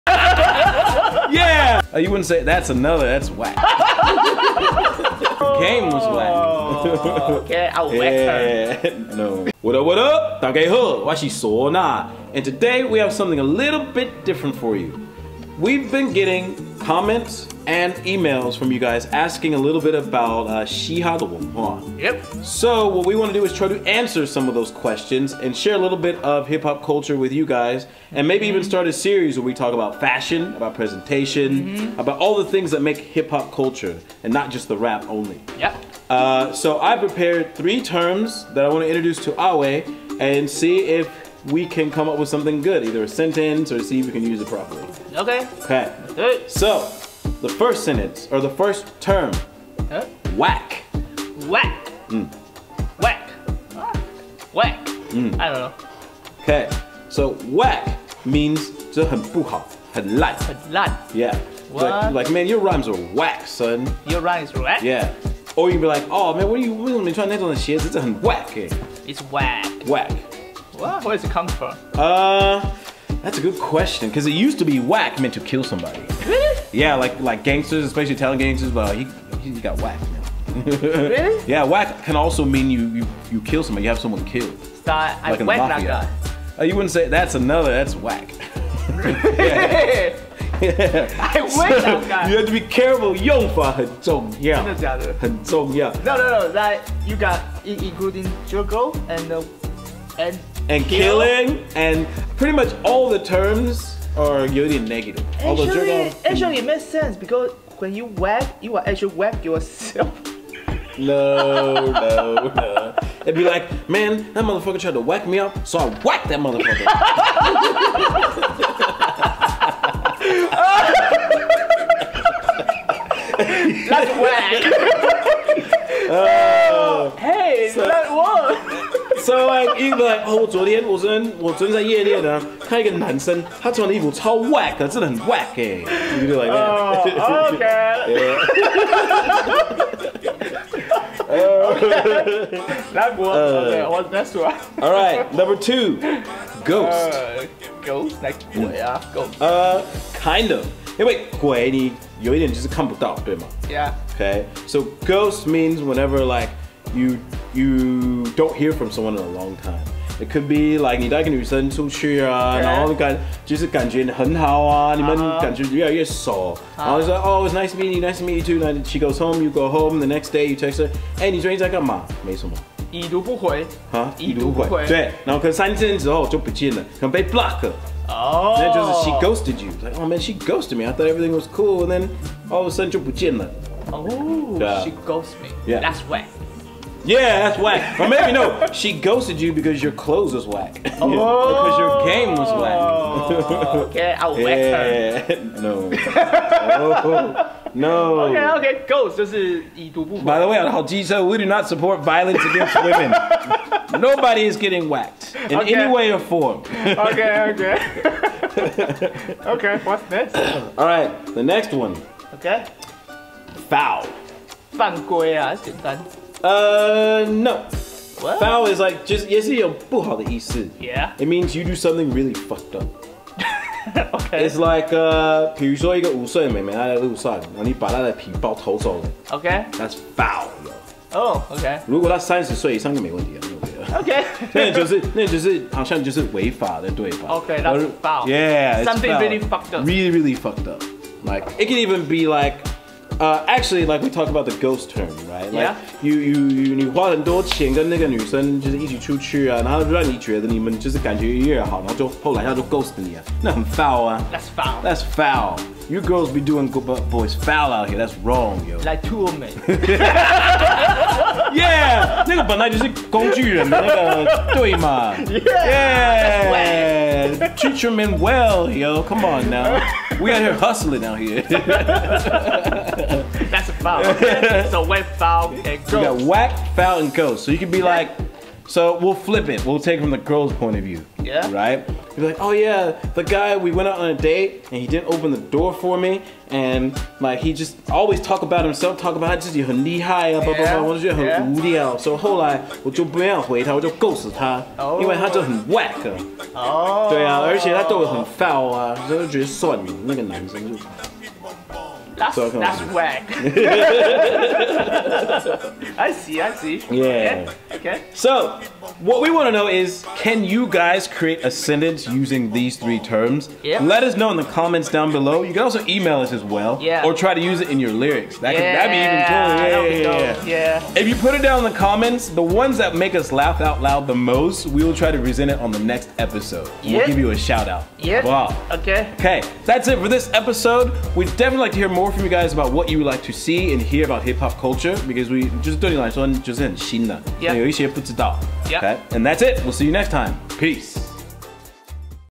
好機車嘻哈英文 想快速長腹肌? 就一起跟嗩吶講英文!
Sona會傳授大家道地的嘻哈用語~
同時也幫助各位快速的長腹肌!一舉兩得!